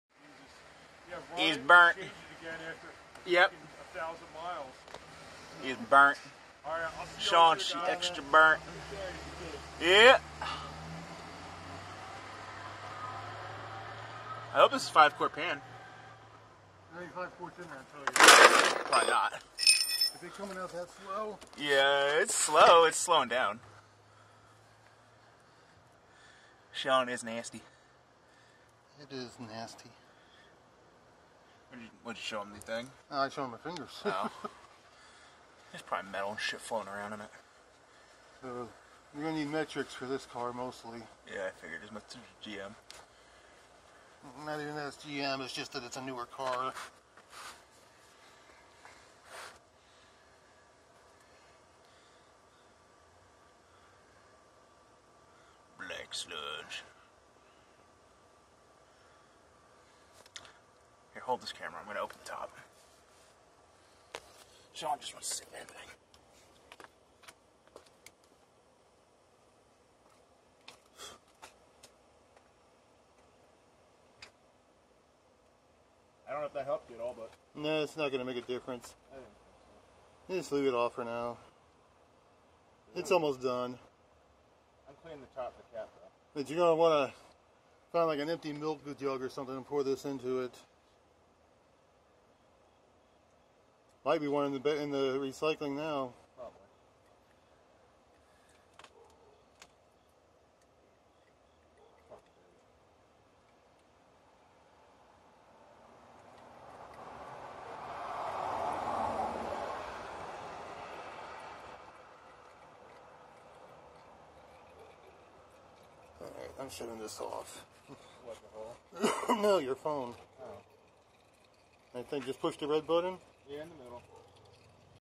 Dumping oil out of my 05 Chevy cobalt
It's burnt